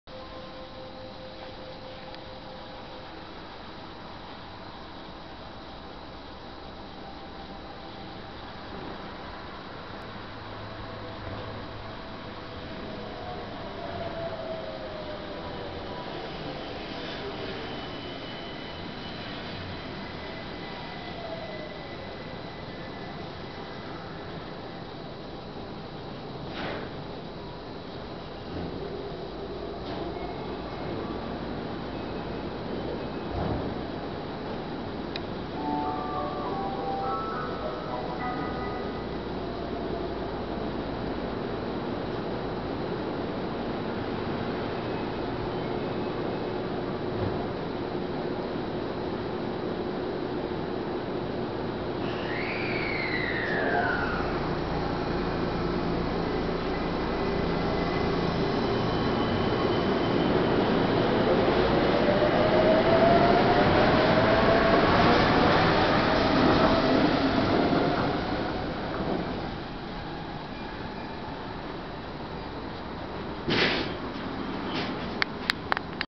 多摩モノレール1000系上北台行を撮った。高松駅
高松駅にて